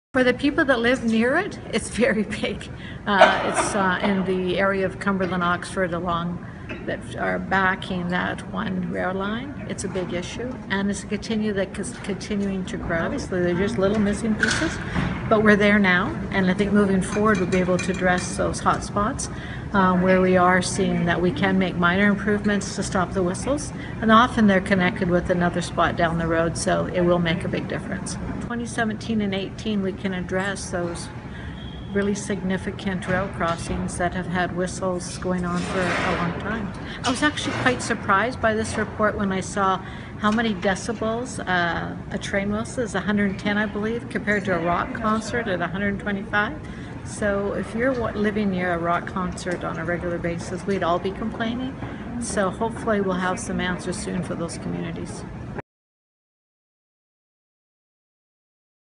Train whistles to be phased out
New safety measures at eight rail crossings could finally stop trains from sounding their horn near residential neighbourhoods. Coun. Bev Esslinger speaks Monday before a committee meeting debate on the issue.